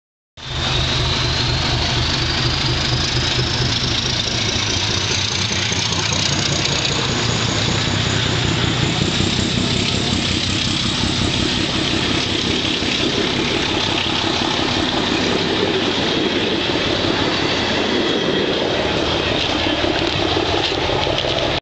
749 107 na čele R 1246 - Zliv rozjezd - 29.1.2011.
749 107 na čele R 1246 odjíždí ze Zlivi do Českých Budějovic